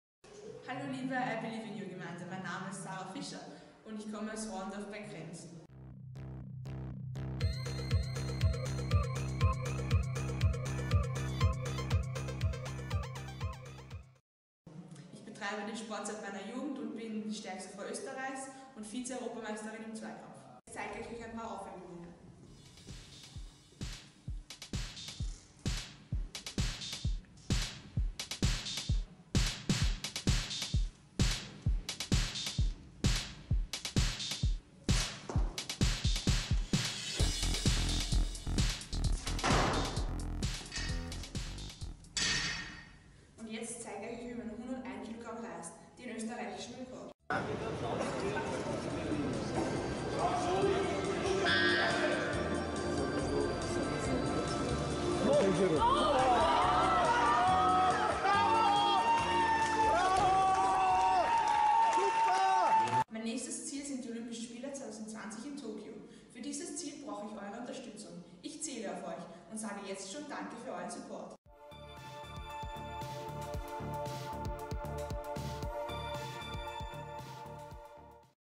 IBIY Projekt von Gewichtheberin Sarah Fischer
Unterstützt mich auf IBIY.at um meinem Ziel - den olympischen Spielen 2020 in Tokyo - einen Schritt näher zu kommen.